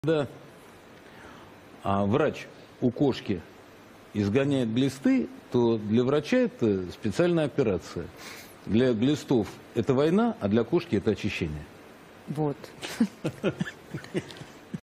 Ukraine is being dewormed by the doctor..Russia!
PUTIN'S FAVORITE COMMENTATOR VERY SUCCINCTLY EXPLAINS WHAT "SPECIAL OPERATIONS" MEANS